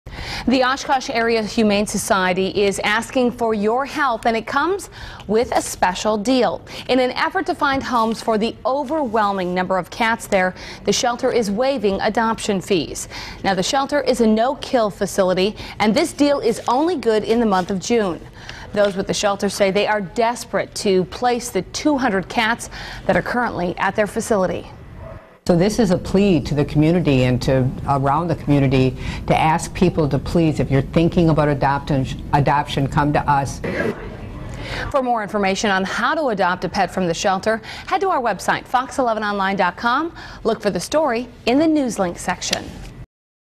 Oshkosh Cat Overflow
The Oshkosh Area Humane Society is asking for your help.  And with it, comes a special deal.